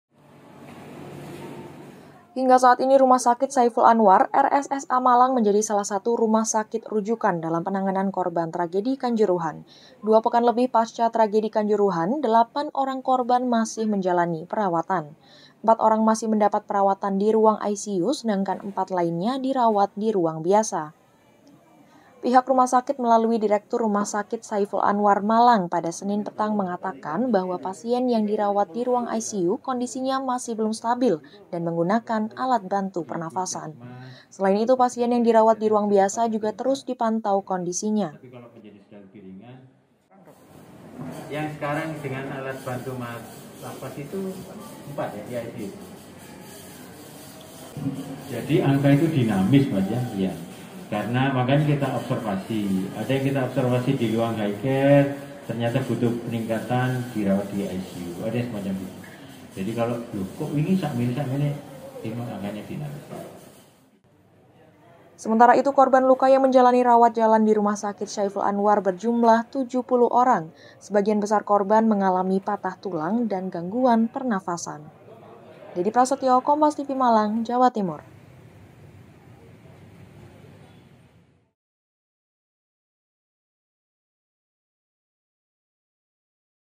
Pasca Tragedi Kanjuruhan, 4 Korban Masih Dirawat di ICU
MALANG, KOMPAS.TV-Hingga saat ini Rumah Sakit Saiful Anwar (RSSA) Malang menjadi salah satu rumah sakit rujukan dalam penangangan korban tragedi Kanjuruhan. Dua pekan lebih pasca tragedi Kanjuruhan, 8 orang korban masih menjalani perawatan. 4 orang masih mendapat perawatan di ruang ICU sedangkan 4 lainnya dirawat di ruang biasa.

Pihak Rumah Sakit melalui direktur RSSA Malang pada Senin (17/10/2022) petang mengatakan bahwa pasien yang dirawat di ruang ICU kondisinya masih belum stabil dan menggunakan alat bantu pernafasan. Selain itu, pasien yang dirawat di ruang biasa juga terus dipantau kondisinya.

"Ada 4 yang tidak stabil kemudian yang lebih stabil tetap harus kita pantau itu empat, yang masih menggunakan alat bantu nafas 4. Angka itu dinamis, makanya kita observasi, ada yang kita observasi di ruang Hi care ternyata butuh peningkatan perawatan di ICU," Terang Kohar, Dirut RSSA Malang.

Sementara itu korban luka yang menjalani rawat jalan di Rumah Sakit Syaiful Anwar berjumlah 70 orang. Sebagian besar korban mengalami patah tulang dan gangguan pernafasan.